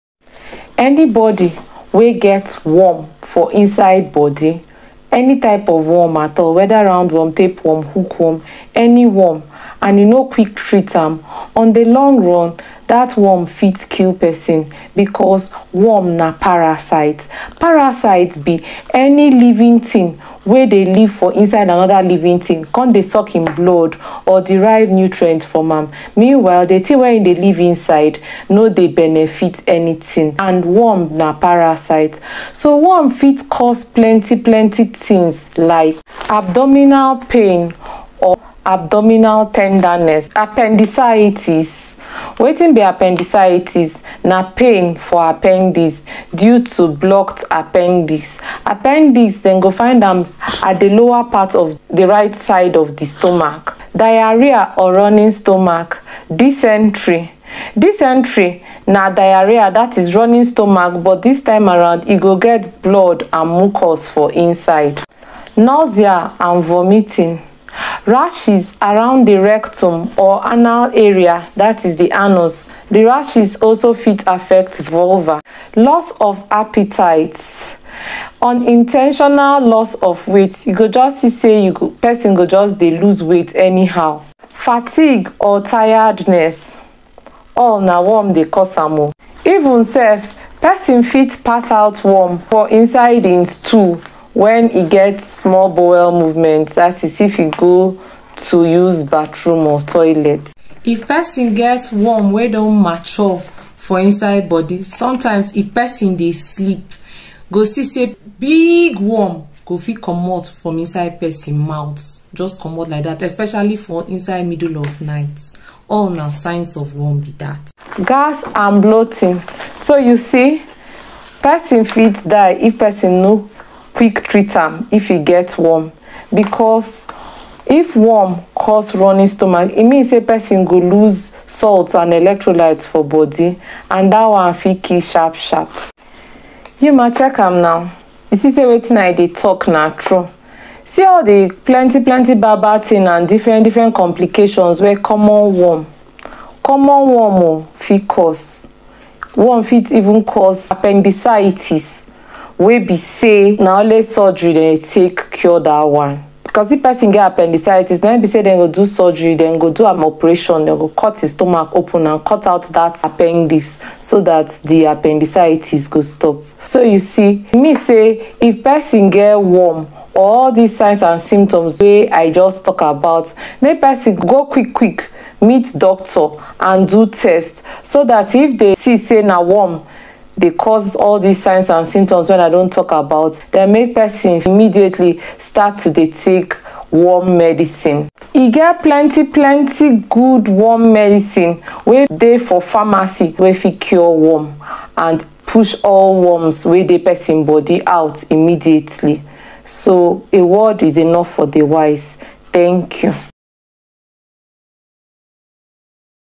Guess what !!! Stomach, intestinal & parasitic worms can kill
Do you know that stomach, intestinal & parasitic worms can kill if not treated? @DrWinnieThePharmacist-Pi-lk5bf